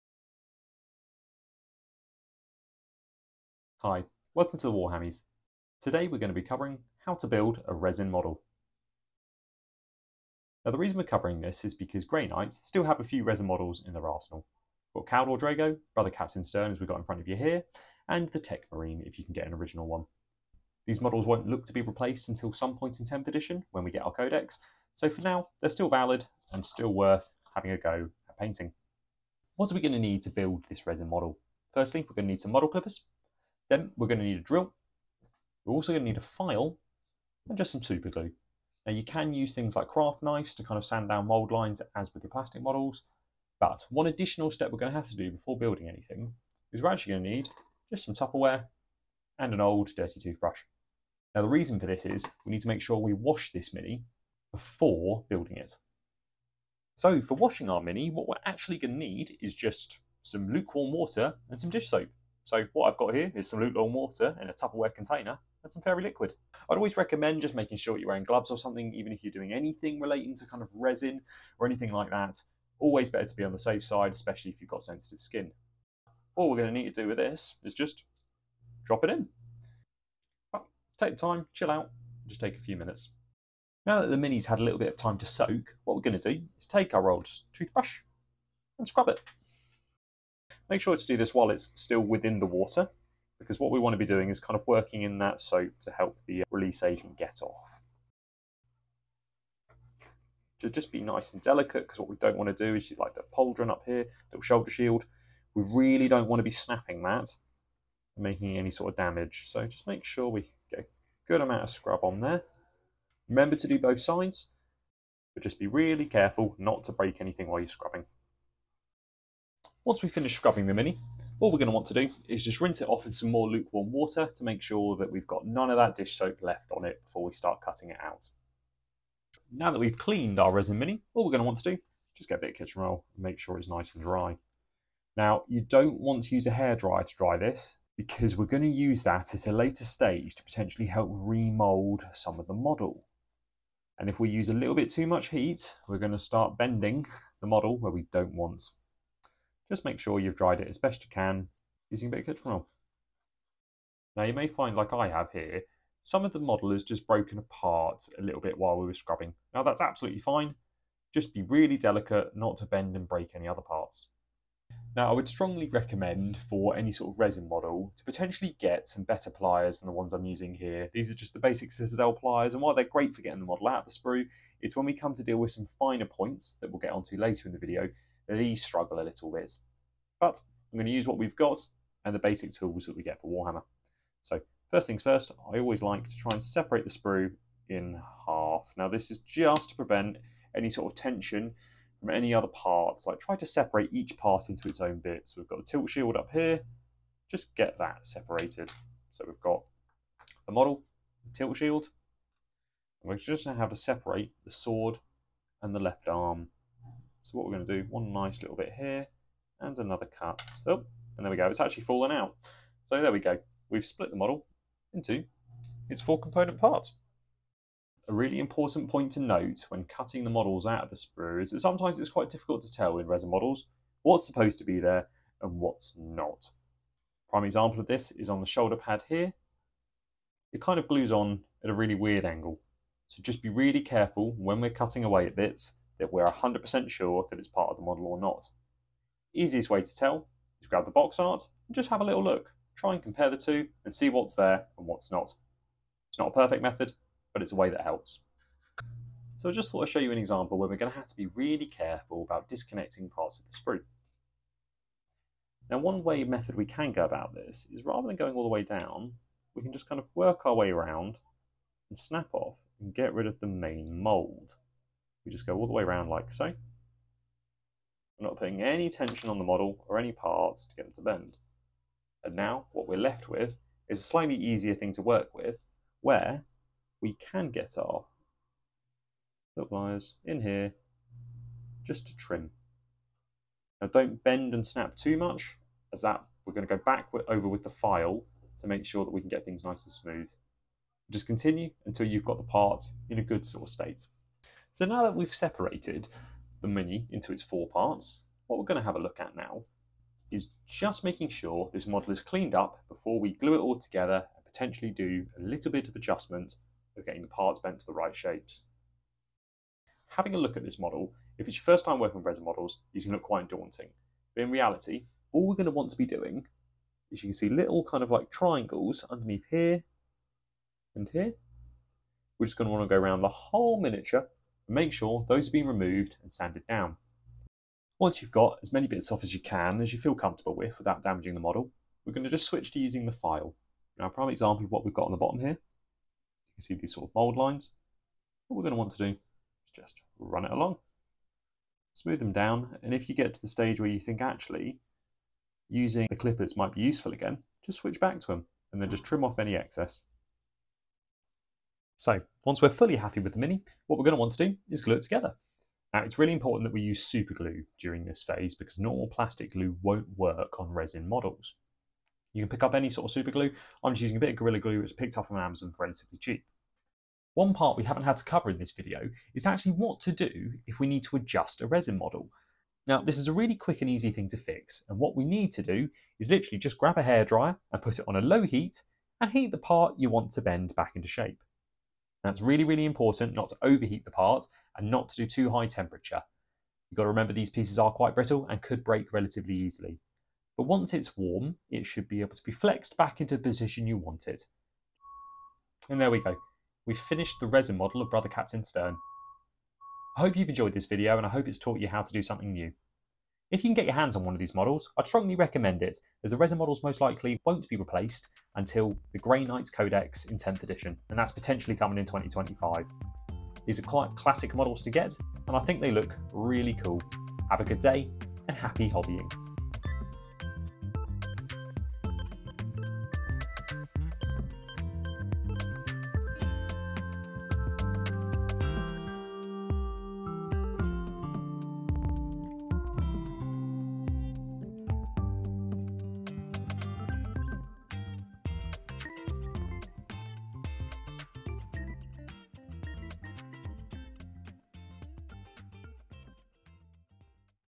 How to Build: Resin Models
In this video, we'll cover how to build a Resin Model, specifically, Brother Captain Stern for the Grey Knights. Resin models are slowly being phased out in 10th Edition but are a fun and interesting challenge to build and paint especially if you want some classic models in your army.

It all helps to support me in making more 40K content that you want to watch!

Happy Hobbying!
To help you navigate the video, please see the chapters below:
0:00 Intro
0:30 What you Need
0:57 Step 1: Washing
2:45 Step 2: Cutting Out
4:55 Step 3: The Details
5:51 Step 4: Glue
6:37 Outro
Please see below for everything that was used during the video (Links provided are for the UK Warhammer online store and some other UK online stores):

Model: Brother Captain Stern - Can be purchased from the Warhammer online store when available as a made to order model.

Tools:
1) Citadel Tools: Model Clippers - These are from the Starter Paint and Tools Set